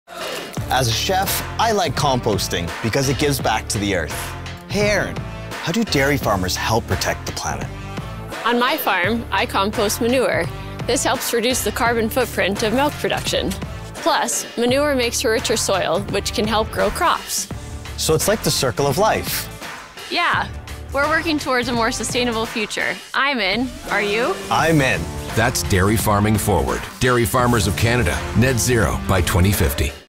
Hey dairy farmer - Sustainability
Canadian Dairy Farmers are committed to reaching net-zero greenhouse gas emissions by 2050. Composting manure helps reduce the carbon footprint of milk production.
dairyfarmersofcanada.ca/en/sustainability